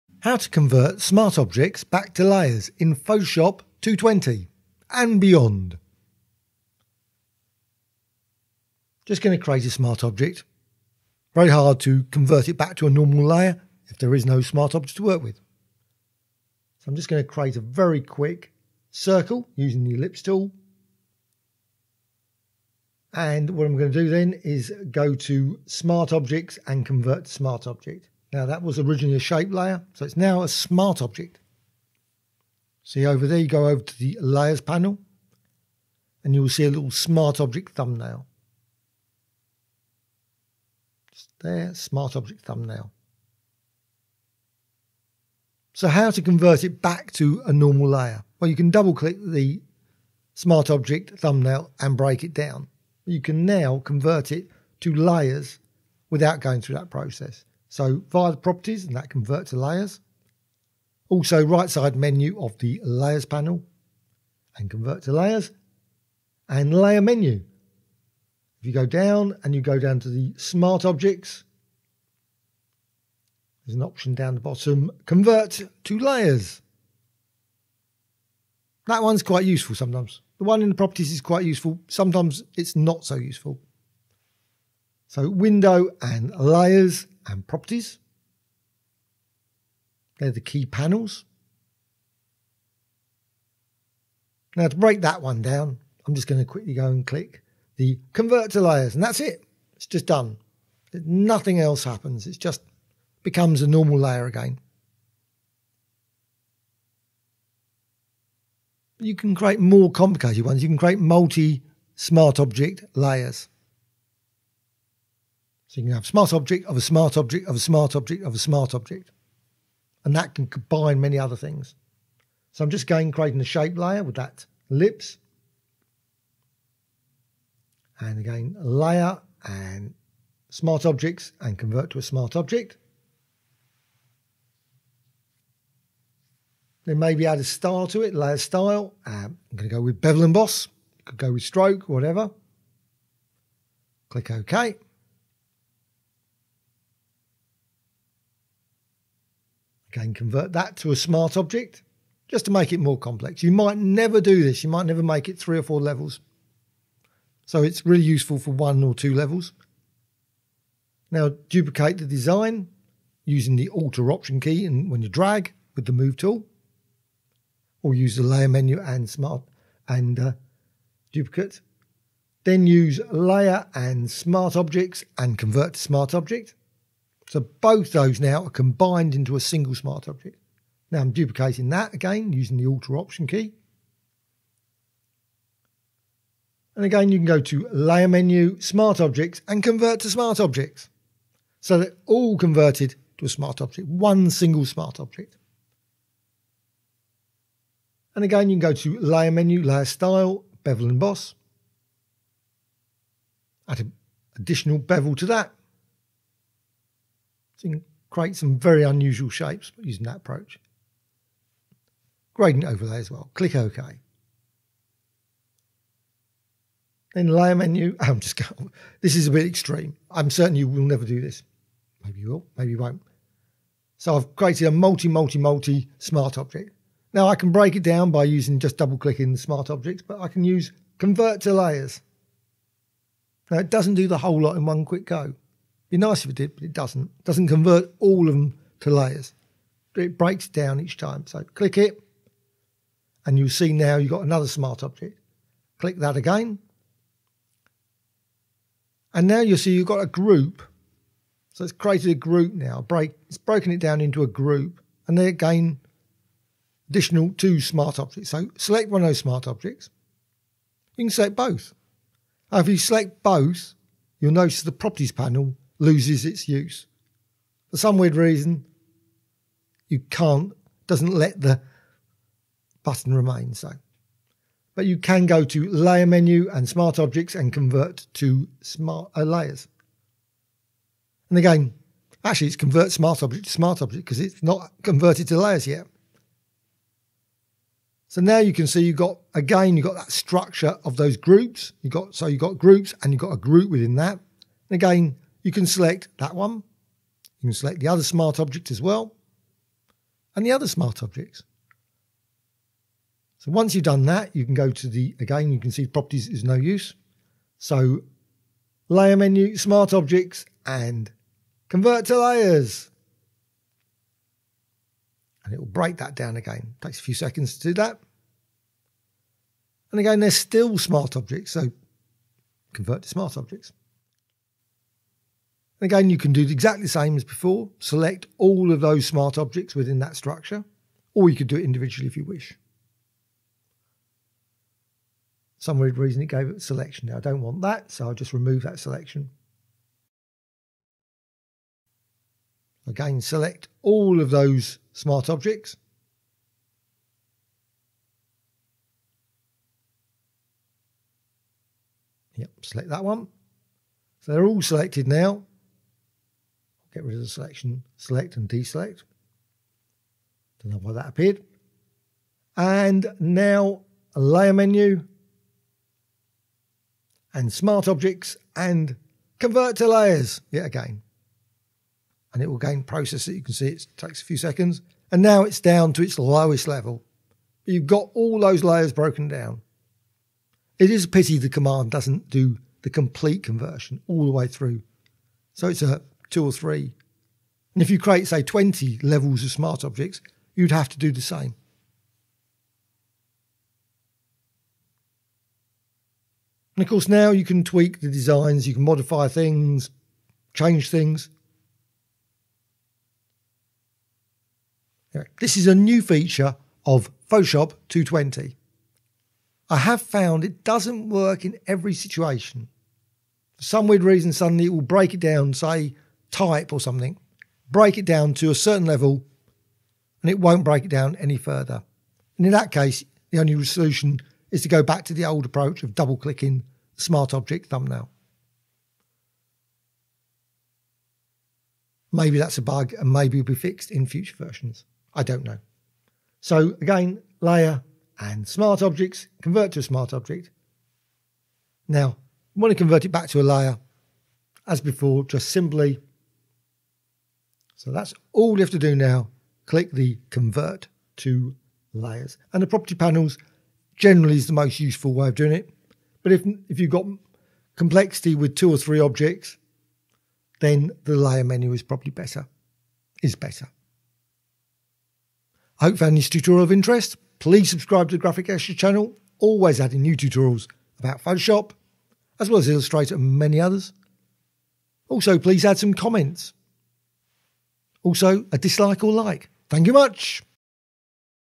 Photoshop : convert smart objects to layers (new feature Photoshop 2020 +)
Photoshop 2020 Convert smart object to layers new feature quick guide - select a smart object and revert it back to its layers / components (as well as repeat reversions to smart objects with multiple smart object groupings). Convert smart objects to layers via three different menus in Photoshop 2020 (and assuming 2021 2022 etc)

This is an update of an earlier (now private) video.